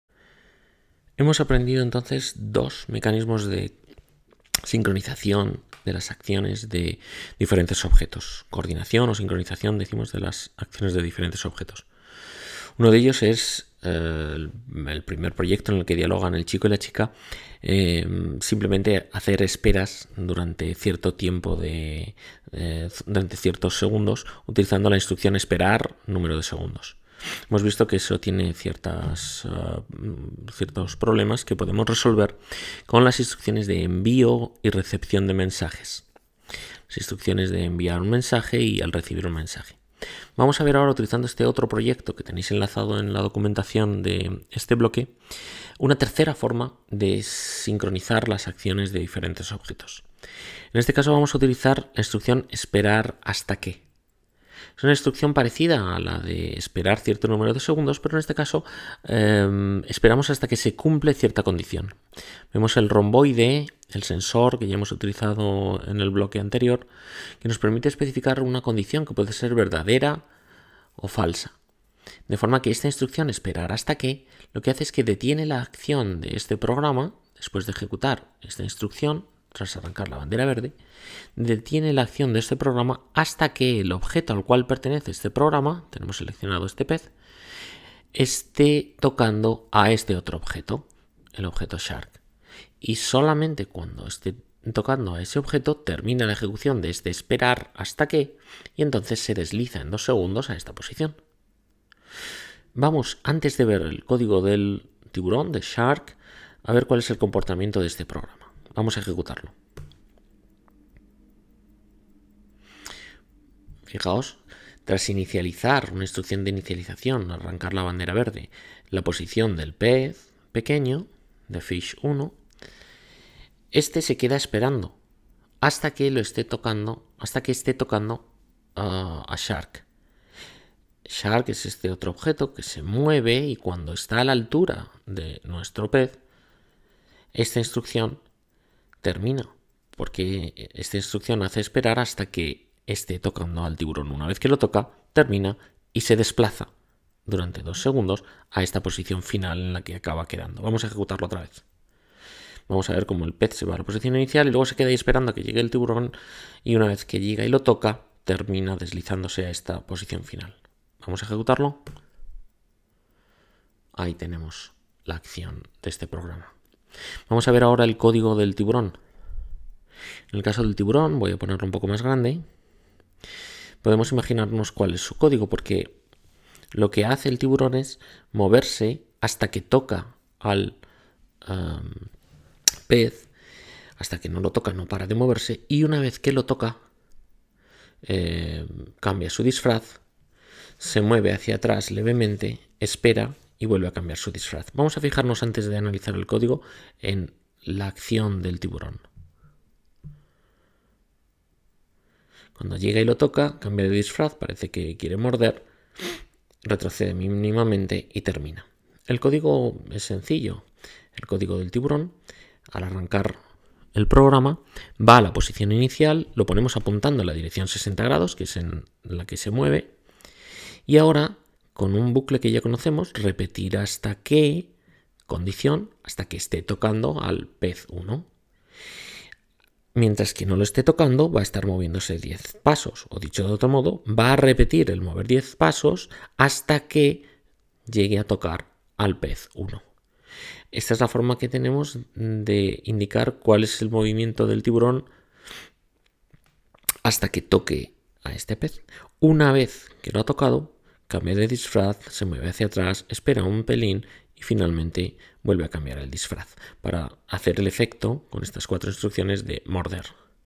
Cómo ejectuar la instrucción 'hasta que'
Vídeo de ayuda para el desarrollo del curso “De espectador a programador: El camino para entender la tecnología aprendiendo a manejarla” de Formación en Red del INTEF